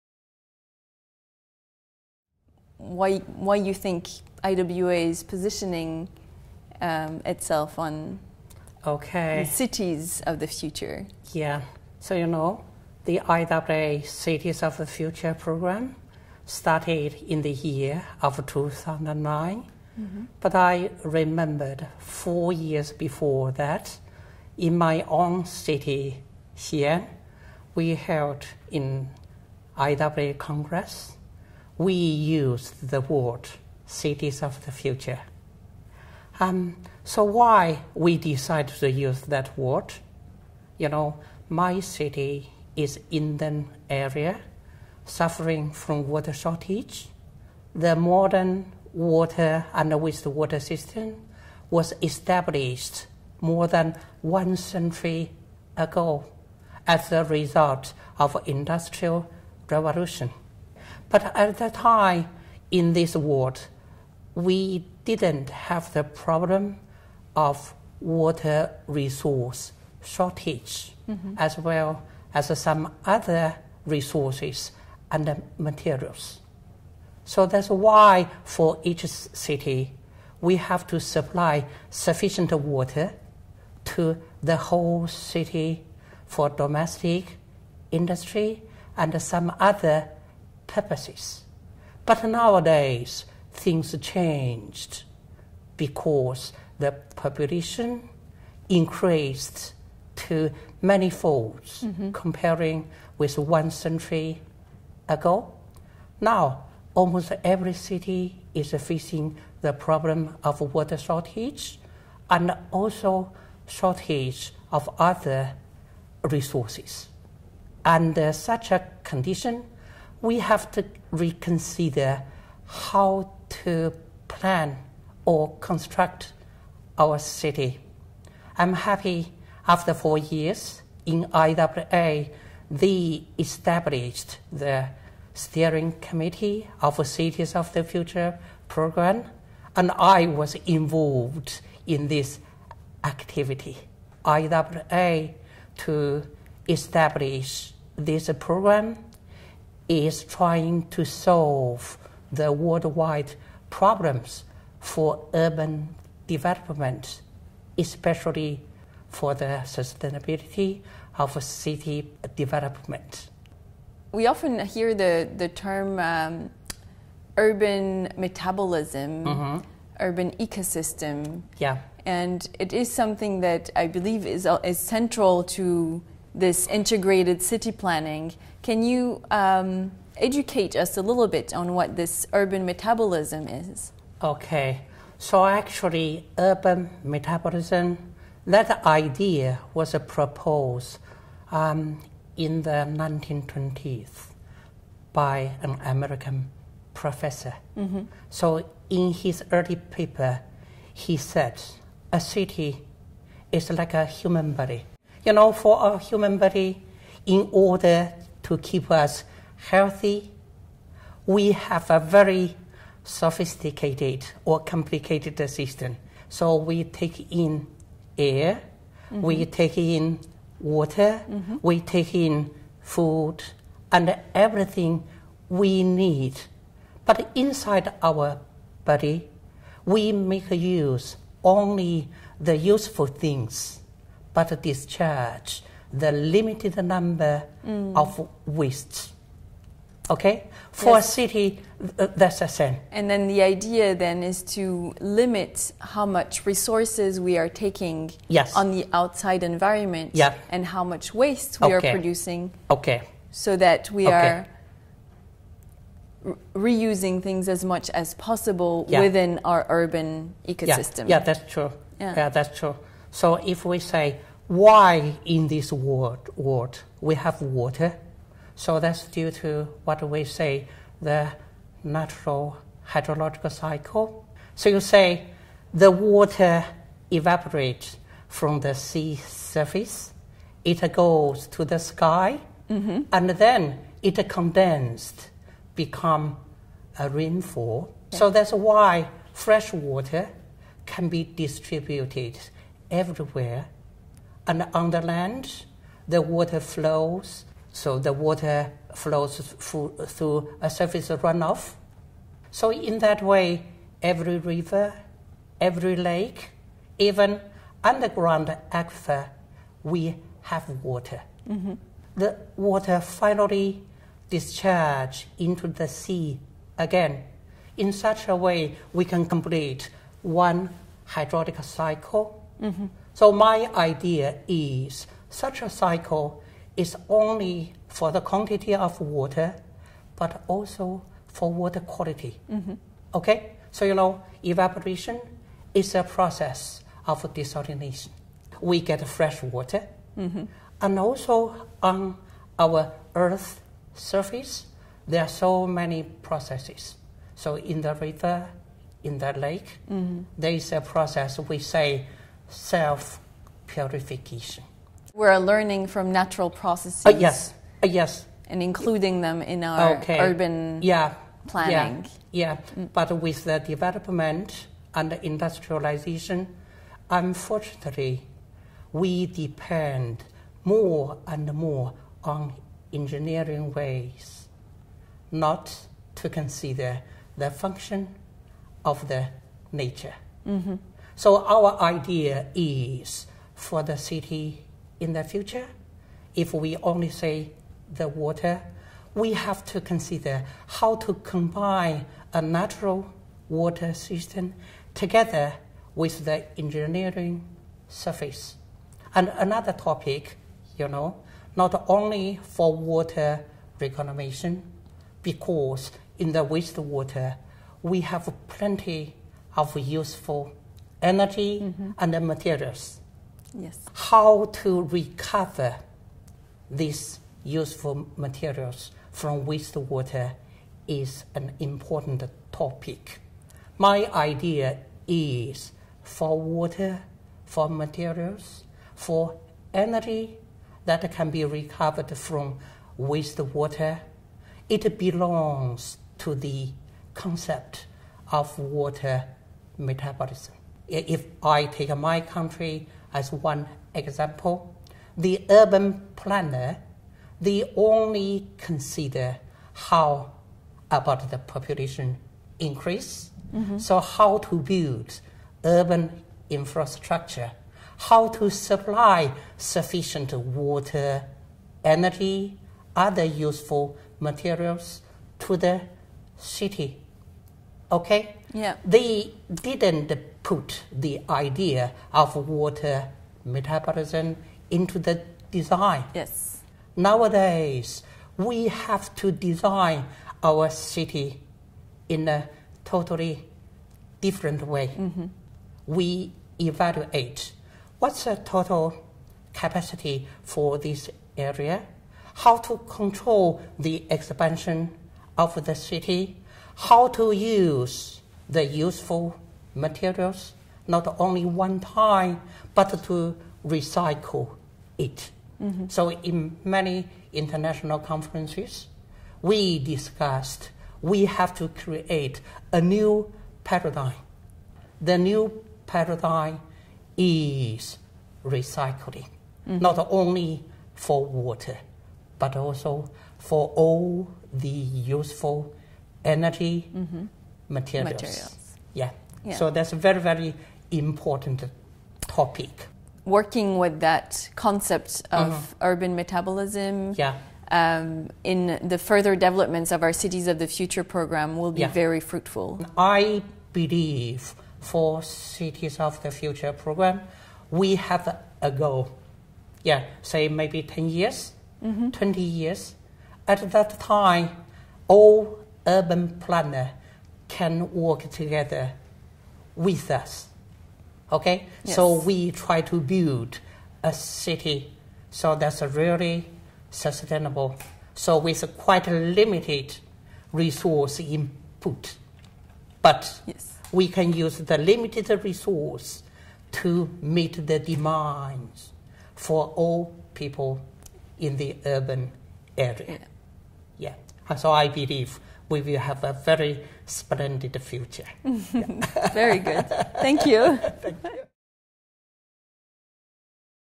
IWA and Cities of the Future
Xiaochang Wang is interviewed by Corinne Trommsdorff, manager of IWA's Cities of the Future program. Xiaochang Wang tell us the story of the programme (2:00), but also about urban metabolism (2:46) and how science learns from natural ecosystems (5:56). The interview ends with the new paradigm for cities (7:00), where resources are limited and recycling is imperative.